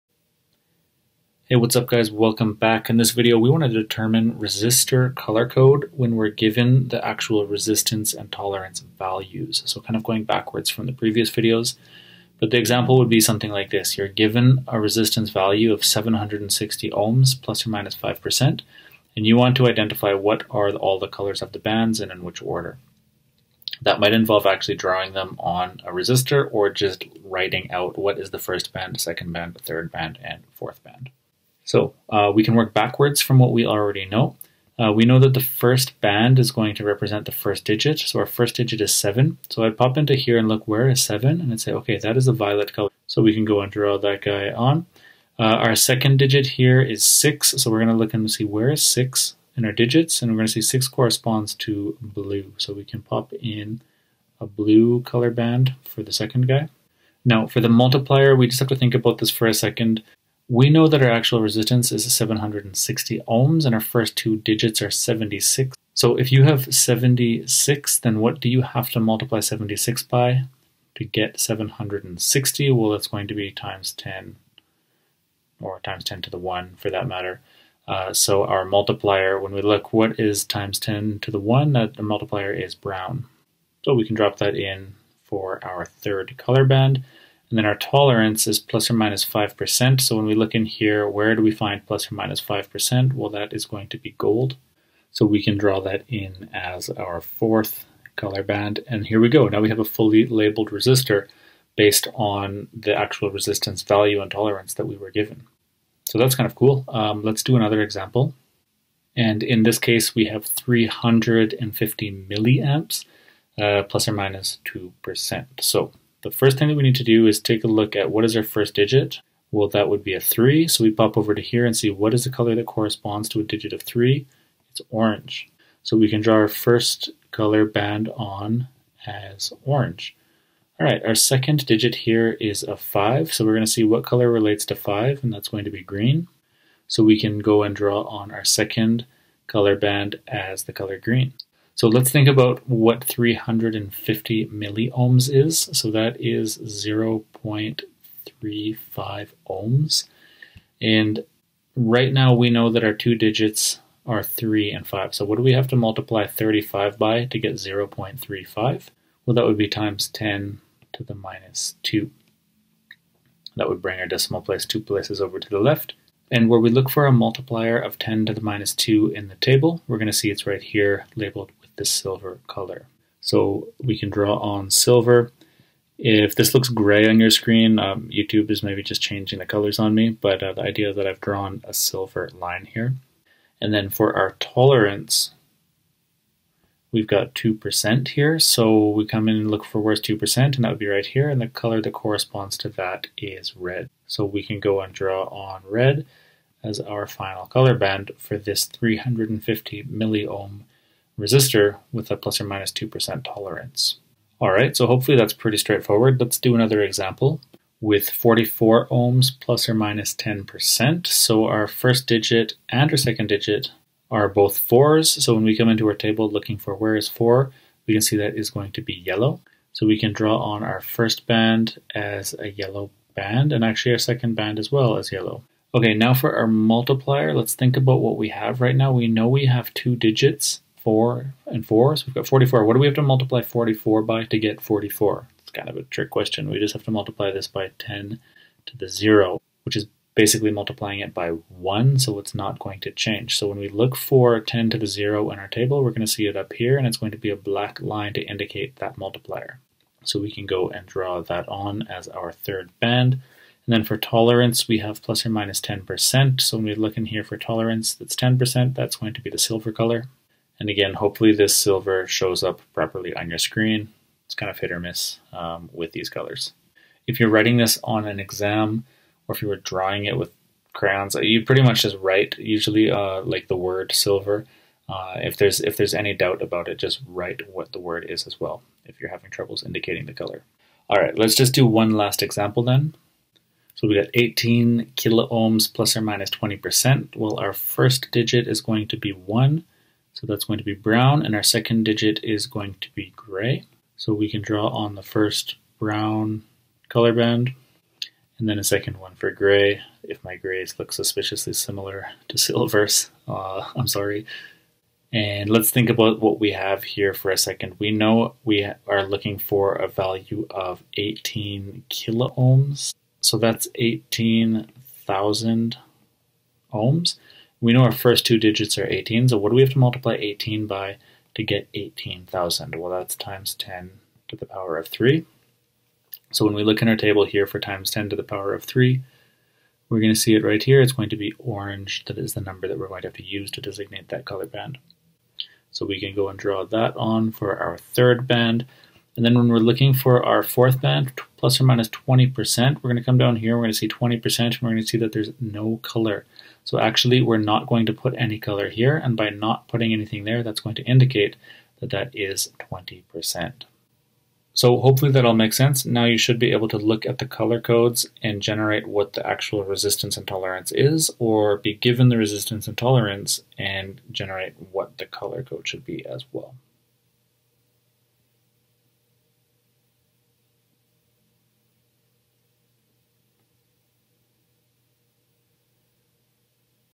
Determine Resistor Colour Code When Given Resistance and Tolerance Values
Resistors use a sequence of 4 colour bands to describe the resistance and tolerance (accuracy) of the resistor. This tutorial goes over some examples in which you are given the resistance and tolerance values, and asked to state what the colour code would be.

This video is part of a full free course on electric circuits. The course covers DC circuits, circuit laws, current & voltage sources, series & parallel resistors, nodal analysis, mesh analysis, and AC circuits.